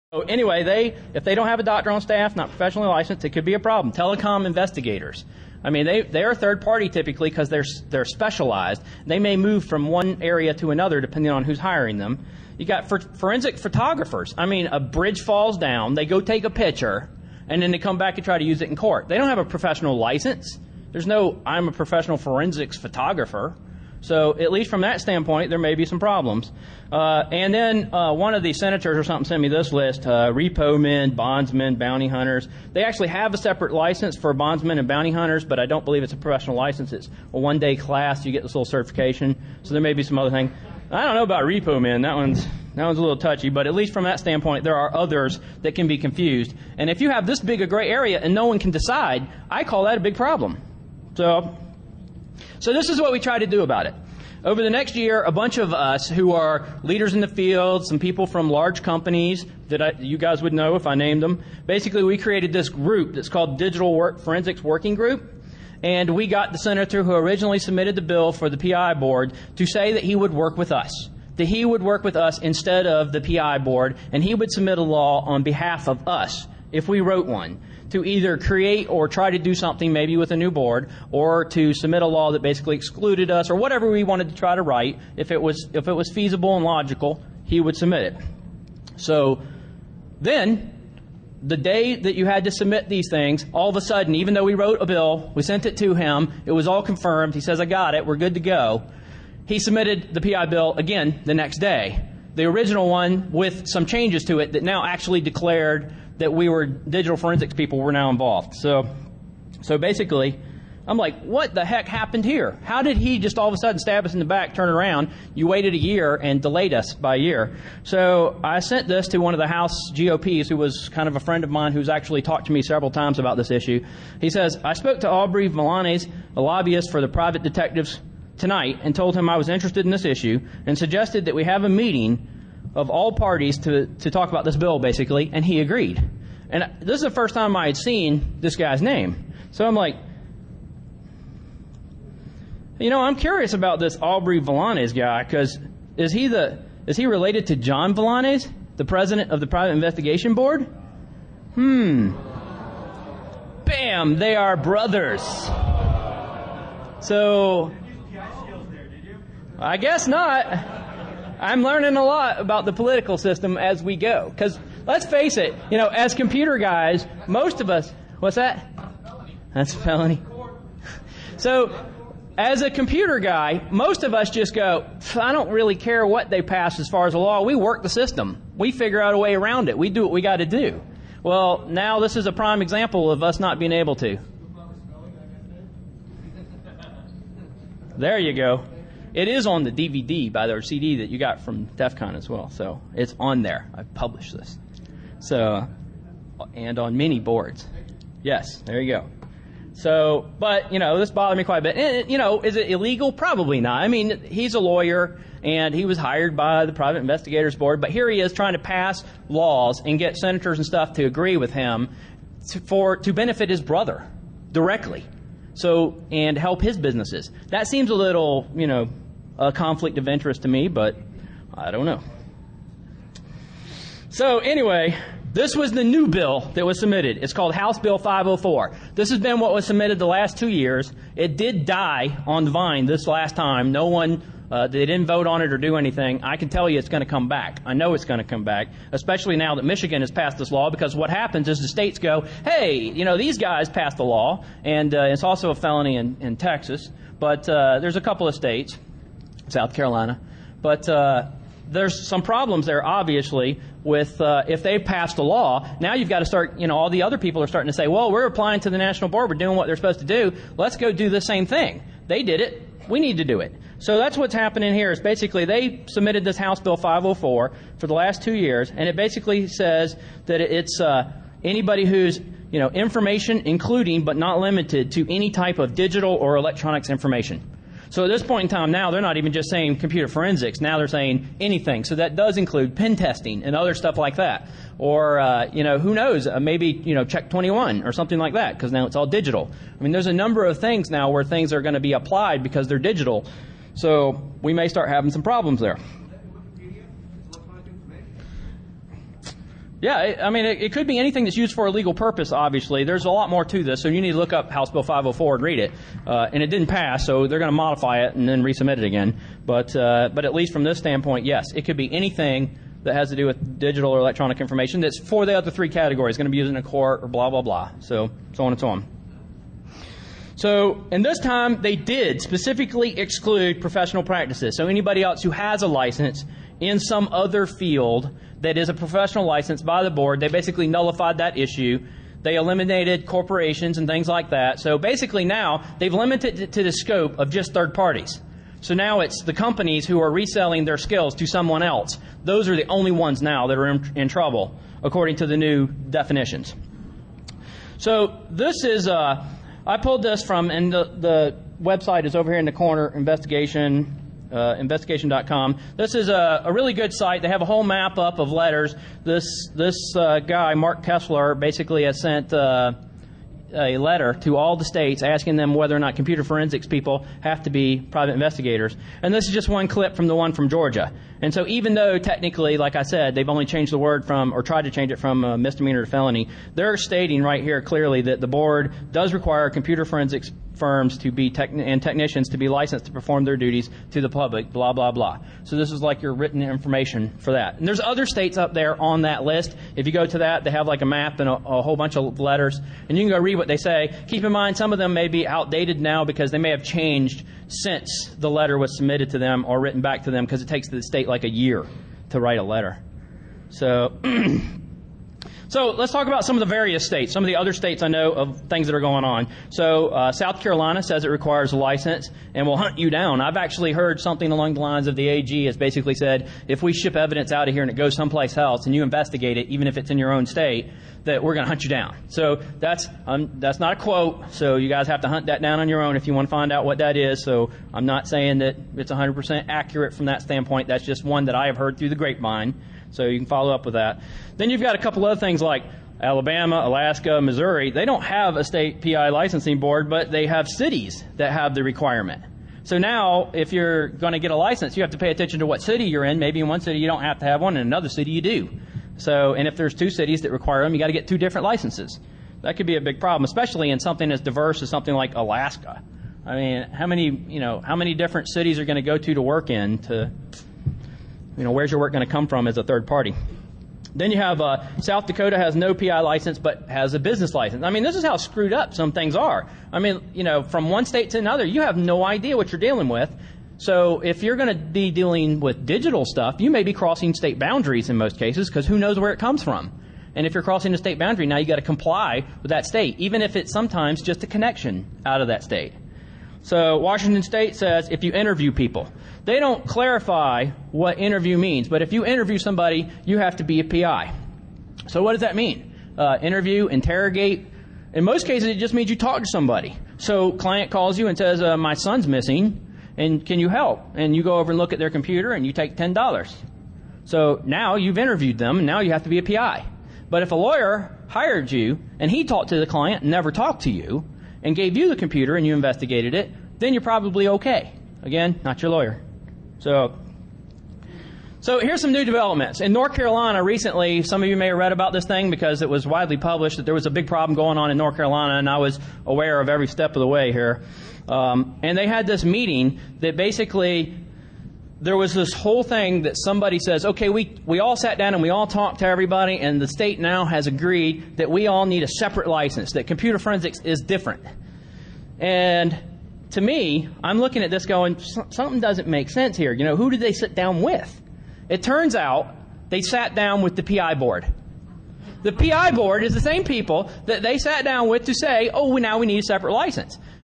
Forensics is only for Private Investigators Part 4 of 6
Each state has been trying to pass laws to prevent computer forensic people from doing their jobs without having a Private Investigator License. Some states have already made it a felony like Michigan.  This is not about Private Investigators enhancing their skills, because this does not change any requirements for a PI to do computer forensics. It only changes YOUR job as a computer forensic specialist by making you work for a PI for two or three years. Your requirements and cost are the only changes.